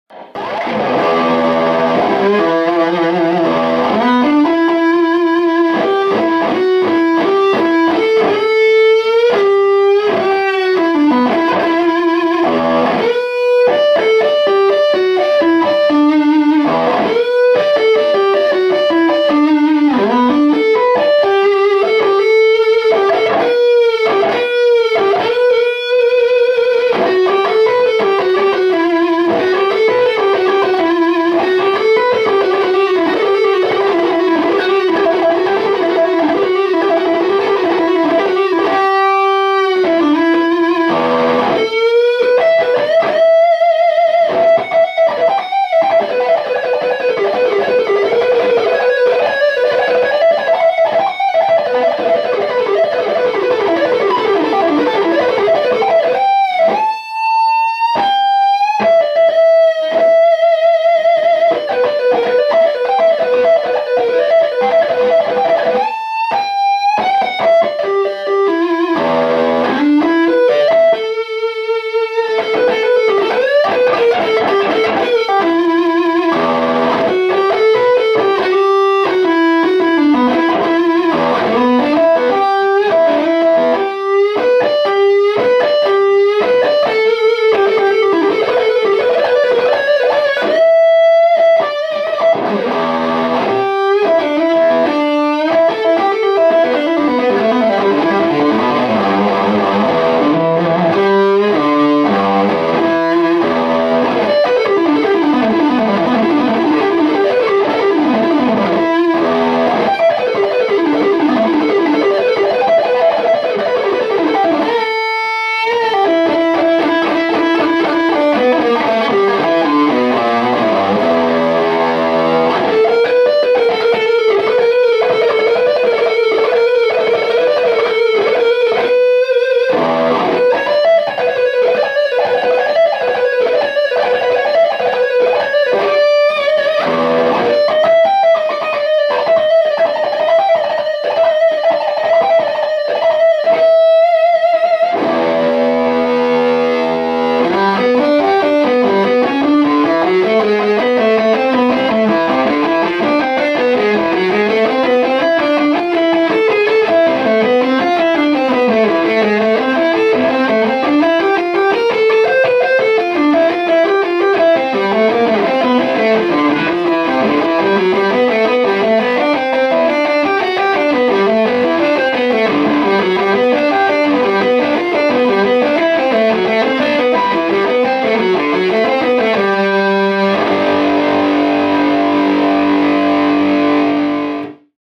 Sam Coulson Rips on Massive ENGL Rig
I'm very proud to announce that I am endorsing ENGL amps.

I'm taking a couple of Powerball II heads and Retro Tube cabs on the road with ASIA this year.

What phenomenal sound!!

If you could take the time to rate the video I will be forever grateful :-)